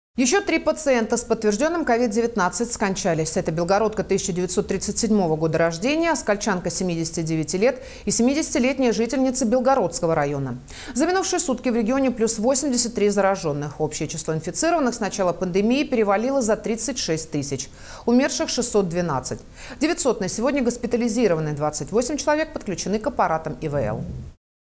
Общее число инфицированных COVID-19 в регионе с начала пандемии перевалило за 36 тысяч
Еще три пациента с подтвержденным коронавирусом скончались. Это белгородка 1937 года рождения, оскольчанка 79-ти лет и 70-летняя жительница Белгородского района. За минувшие сутки в регионе плюс 83 зараженных. Общее число инфицированных с начала пандемии перевалило за 36 тысяч. Умерших - 612. 900 на сегодня госпитализированы. 28 подключены к аппаратам ИВЛ.